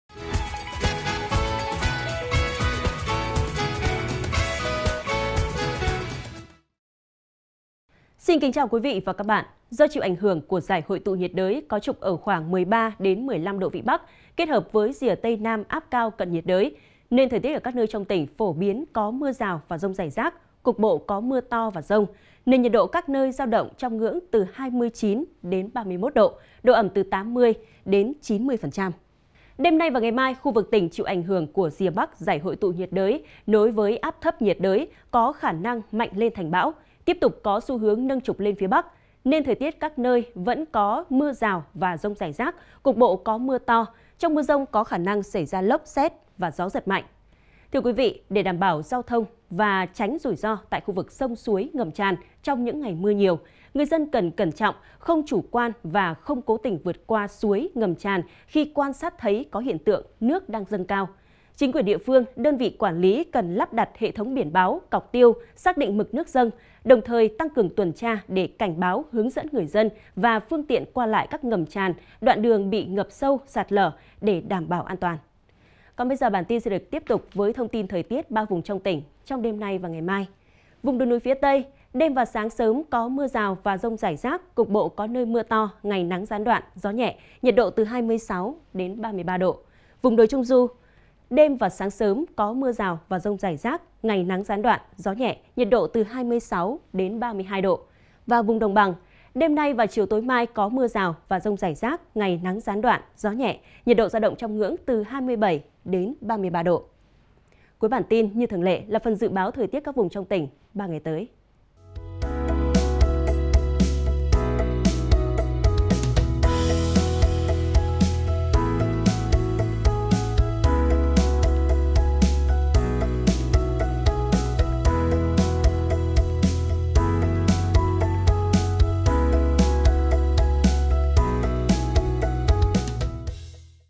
Thời tiết Phú Thọ ngày 21/7/2024
Thời tiết Phú Thọ ngày 21/7/2024, thông tin thời tiết 3 vùng trong tỉnh